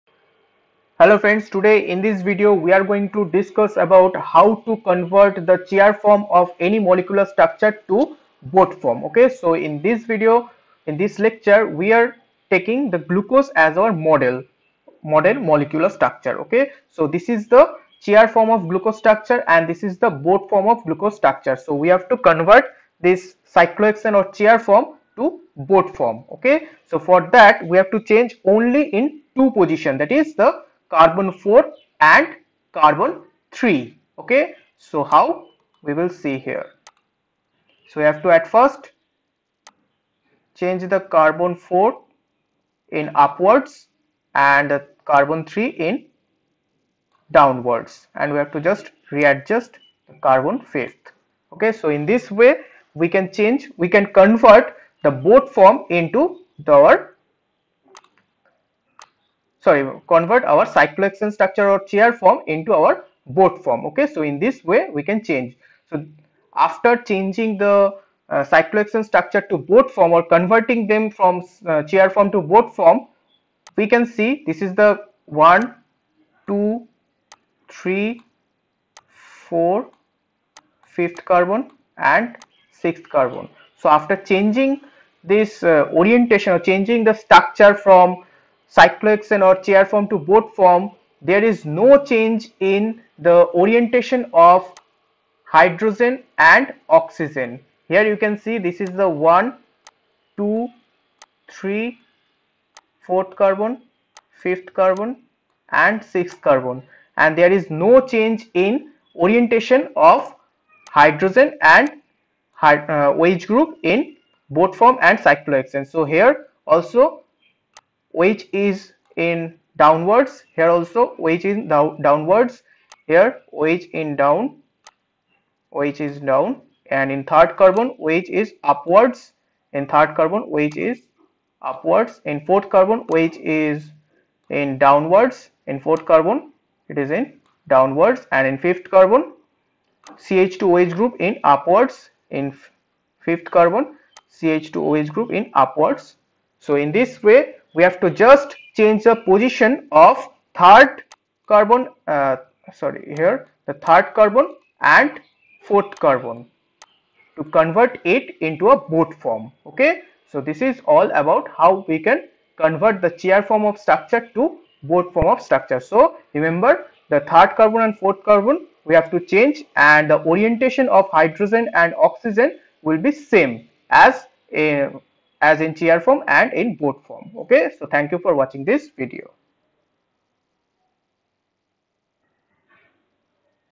HOW TO CONVERT CHAIR CONFORMATION TO BOAT CONFORMATION OF GLUCOSE | CHAIR FORM TO BOAT FORM
the conversion between Fischer, Haworth, and Chair forms of carbohydrates as well the mechanism that leads to the formation of ɑ-D-glucose and β-D-glucose from the open form of glucose.

We mentioned the formation of the cyclic forms ɑ-D-glucose and β-D-glucose last time, however, did not pay close attention to how the Fischer forms are transformed into the cyclic anomers of glucose.